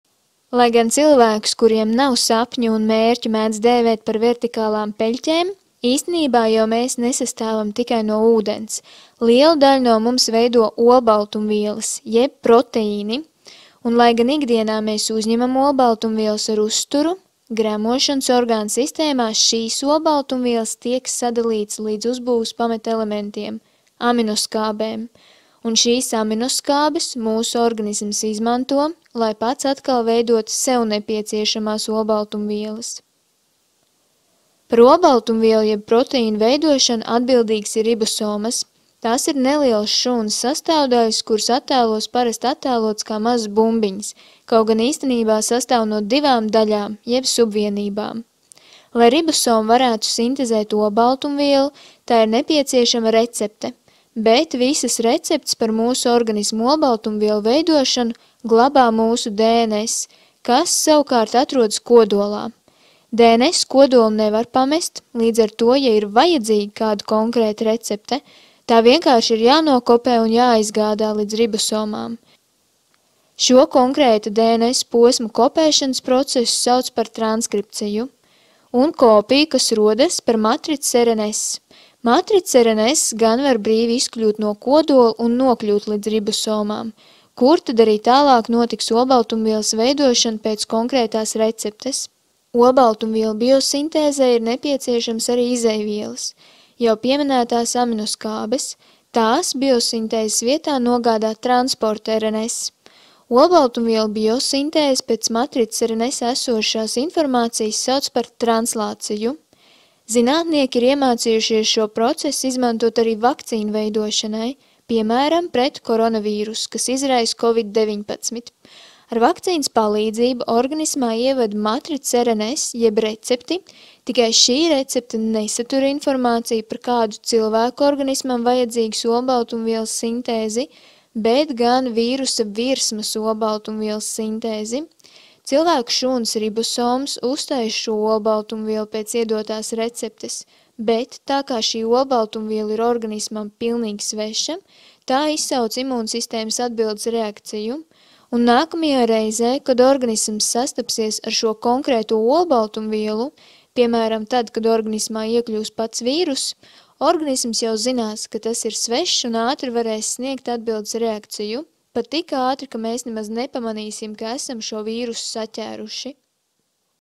Olbaltumvielu biosintēze
Olbaltumvielas jeb proteīni ir svarīga mūsu ķermeņa sastāvdaļa. Video nelielam priekšstatam, kā olbaltumvielas mūsu organismā tiek sintezētas.

Video izmantotie materiāli:
Aminoskābe - By GYassineMrabetTalk✉ This W3C-unspecified vector image was created with Inkscape. - Paša darbs, Neaizsargāts darbs,